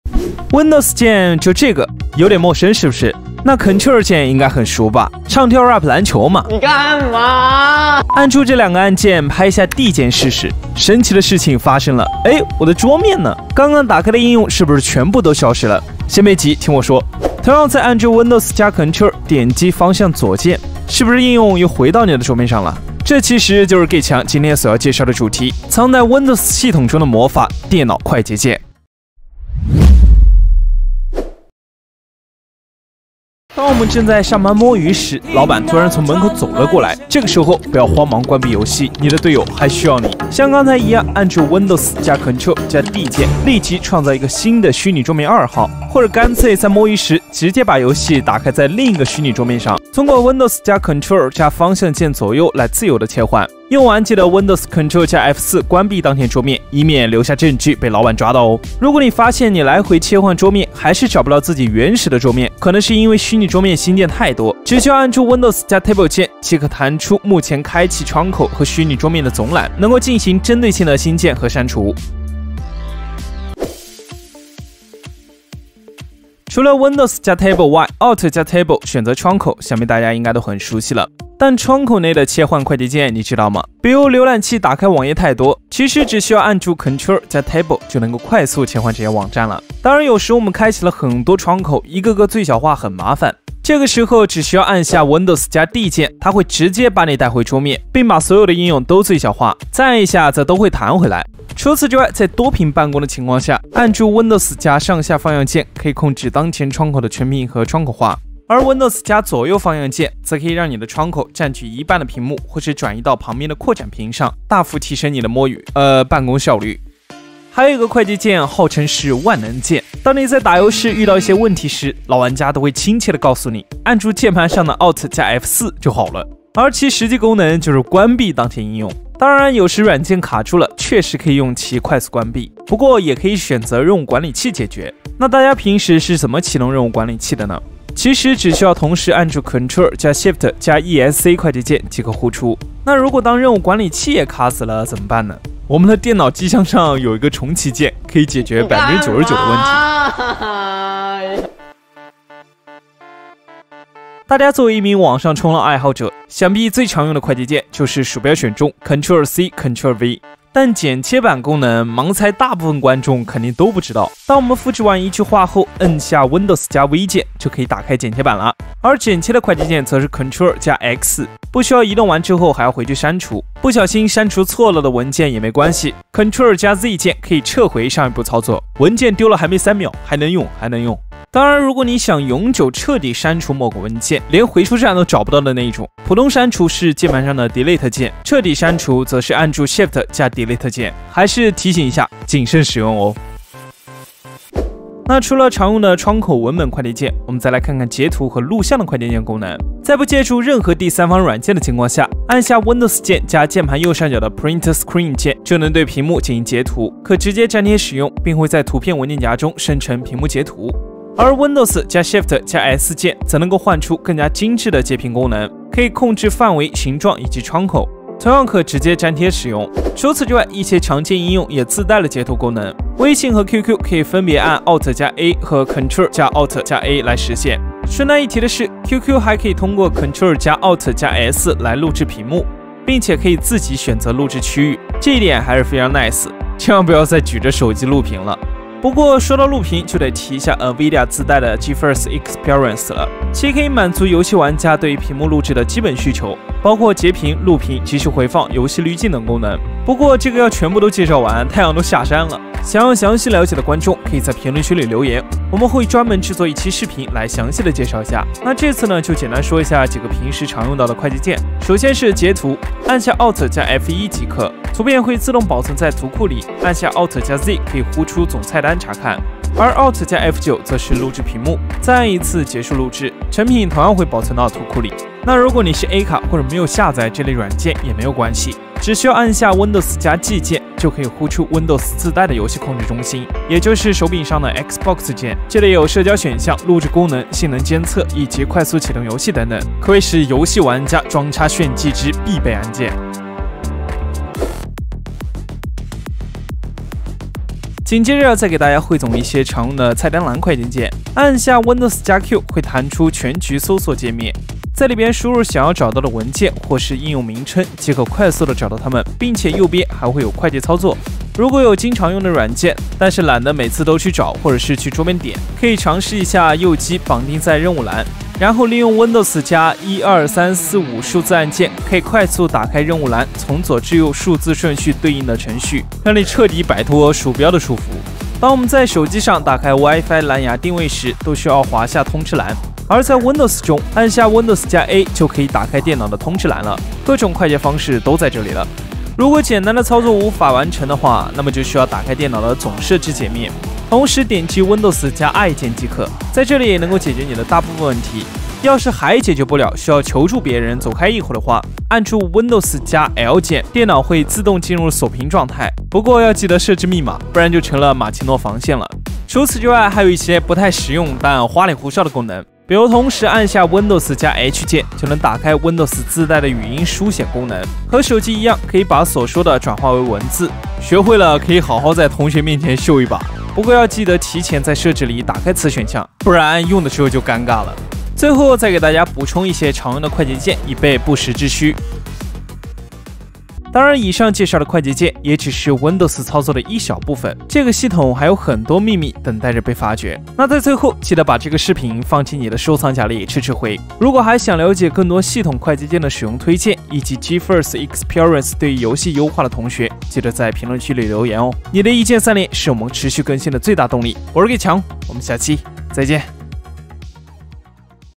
收藏警告！快捷鍵實用大彙總，遊戲摸魚、上班開黑之必備技能「超極氪」
Windows快捷鍵大彙總，面向辦公，摸魚，
打遊戲全方面快捷鍵介紹，幫妳徹底擺脫鼠標的束縛，
沒有繁瑣的操作，再也不用舉著手機錄屏，強烈推薦收藏。
快捷鍵 收藏 必備技能 本頻道製作的所有影片都符合平台的使用政策，沒有任何違規行為。